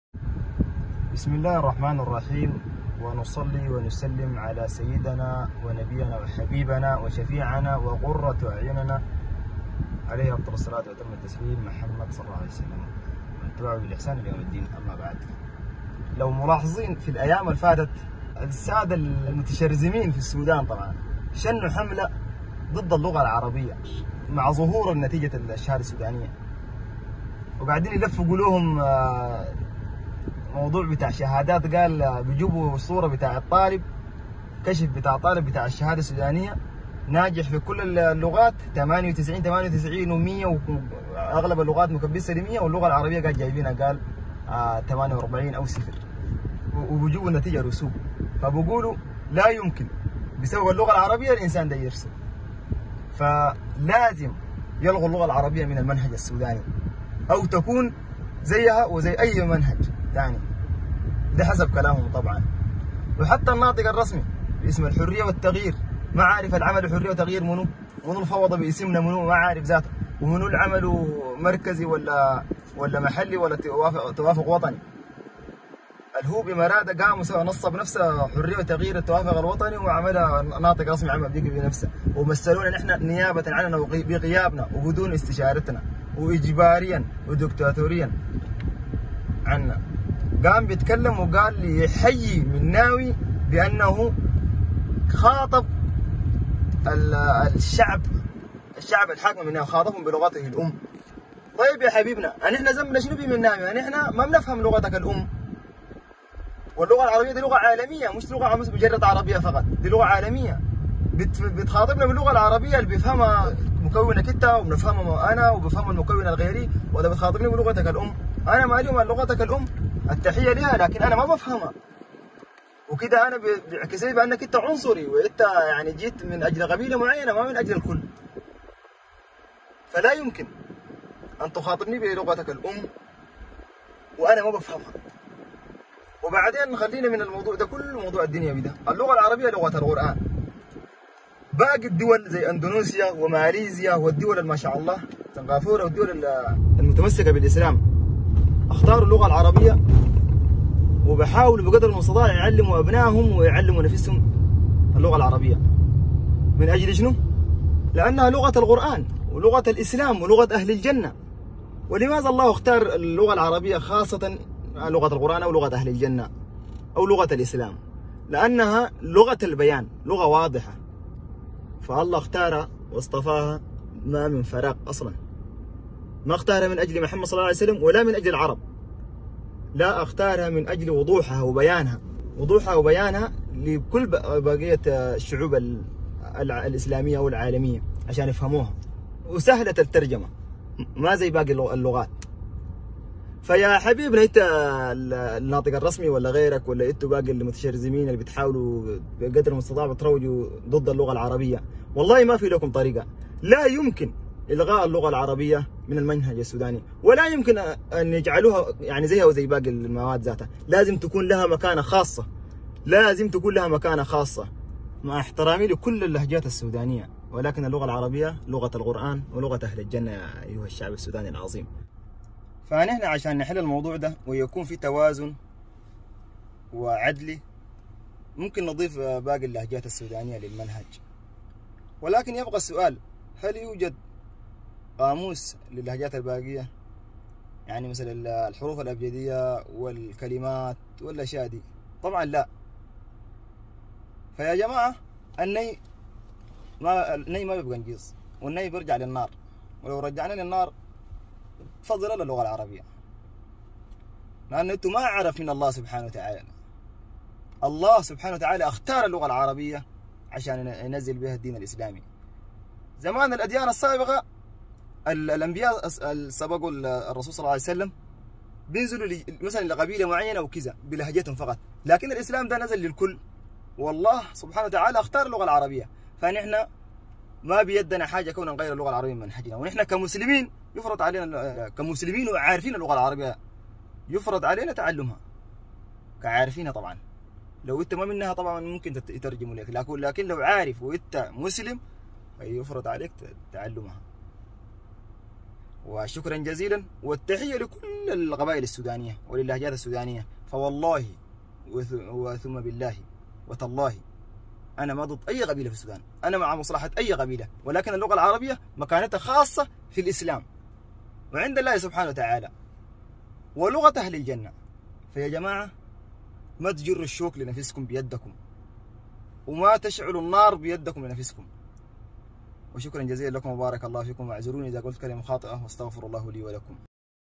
لماذا يهاجمون اللغه العربية ويطالبون بألغائها من المناهج في هذه الدوله
شاهد_بالفيديو في هذا الفيديو نتحدث عن الحمله الحصلت اخيرا ضد اللغه العربية والذين يطالبون بالألغائها من المنهج السوداني وكل هذه التساؤلات نجاوب عنها في هذا الفيديو بإذن الله تعالى